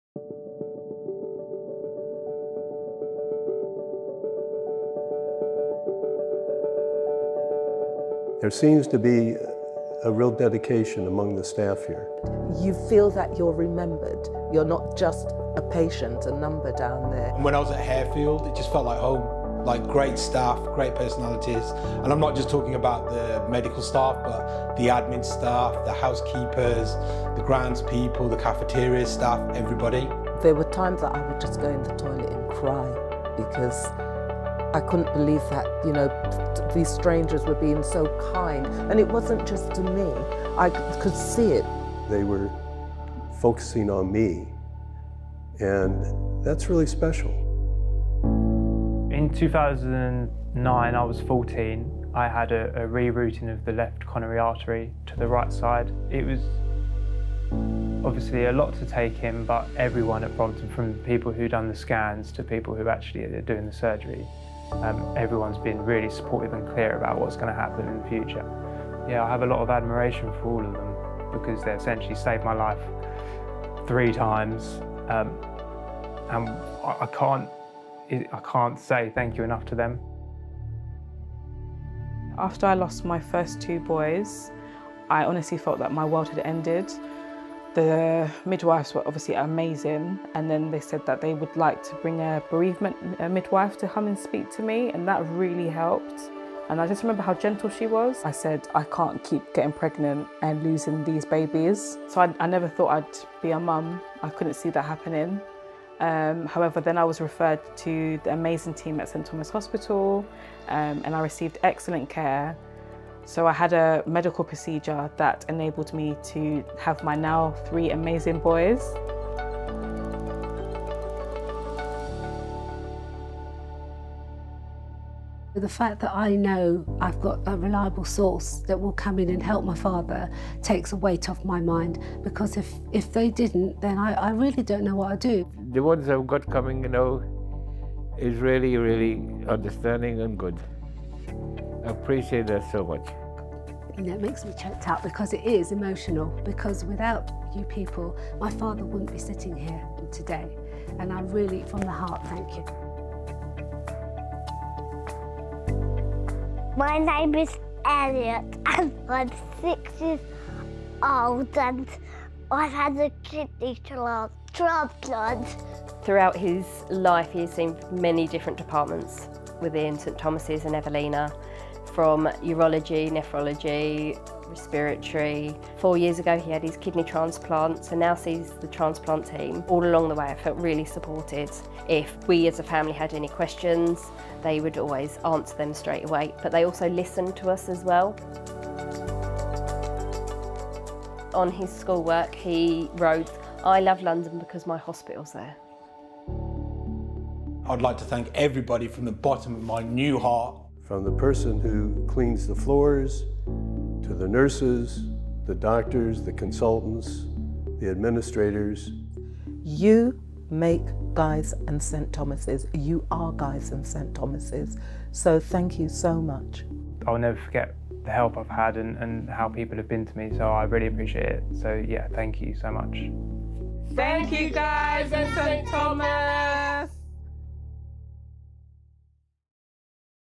Without you...a thank you from the people whose lives we change
In this film some of our patients tell their stories and say thank you to our staff.